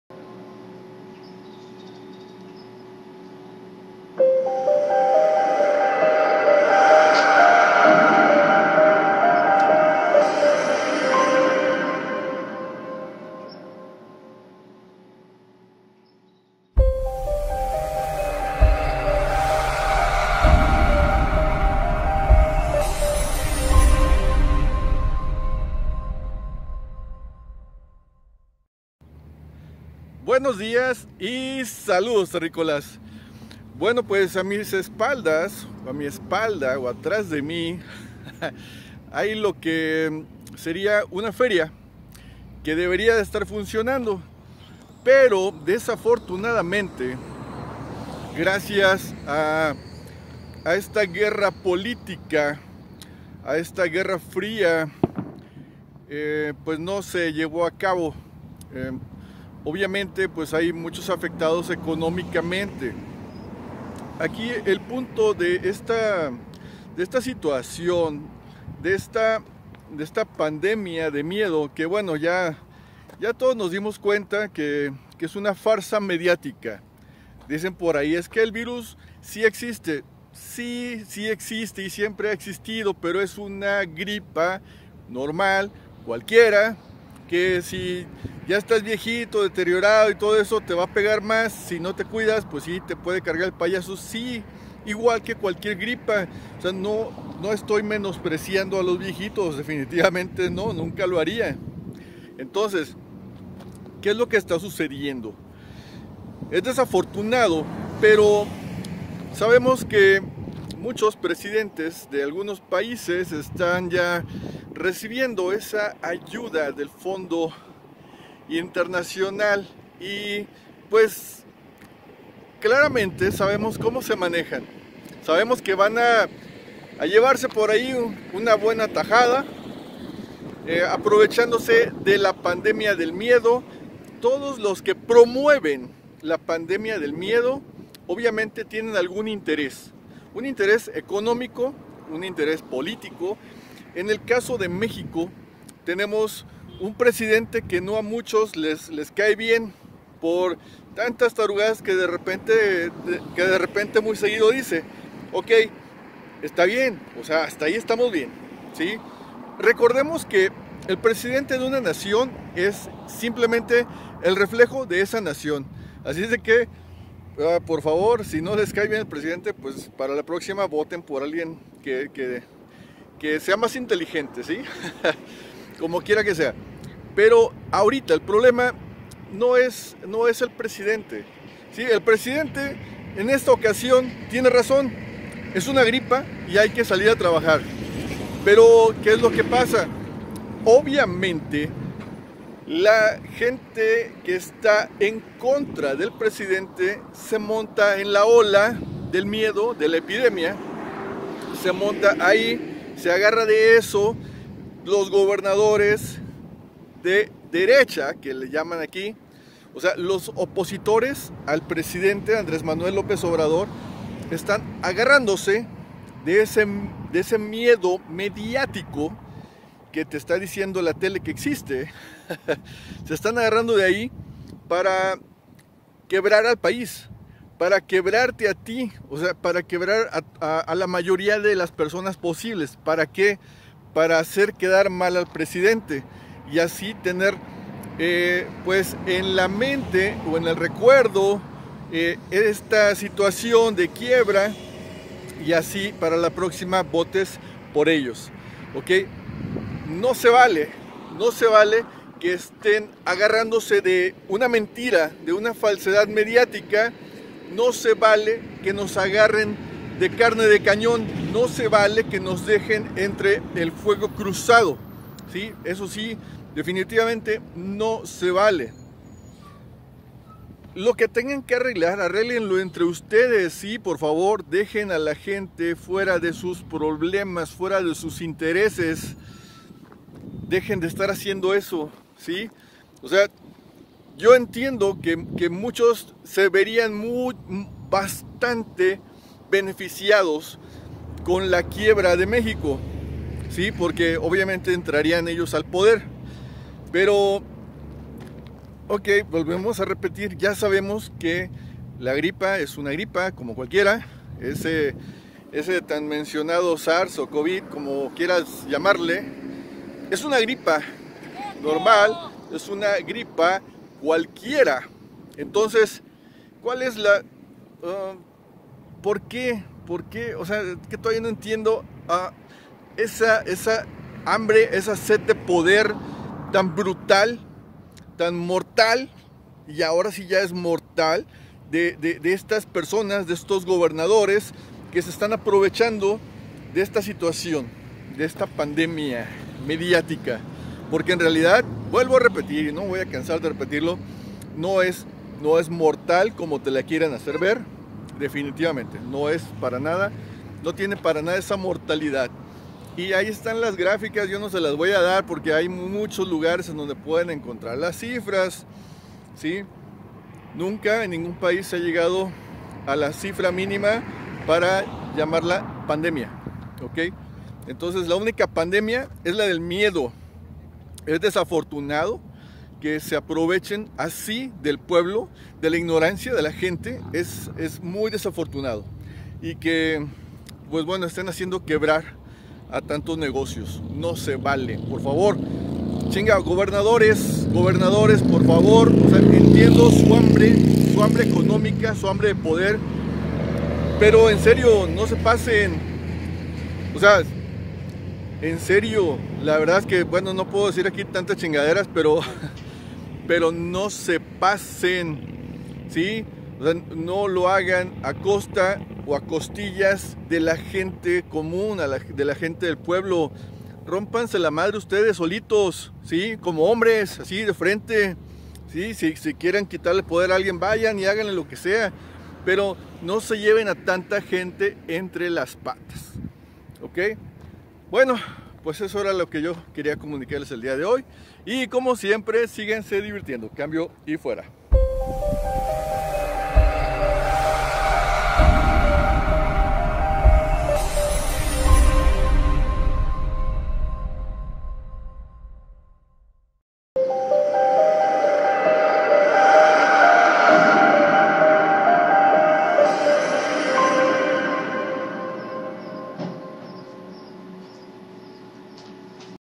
La guerra de las mentiras
La política mundial siempre ha utilizado la mentira para sus propios intereses... esta nueva guerra mediática también está plagada de mentiras y verdades a medias...

Todo es Posible ... con la Herramienta Adecuada y un Verdadero Deseo de Lograrlo!!!

"La Salud; el Bienestar es un Acto de Consciencia"
Face: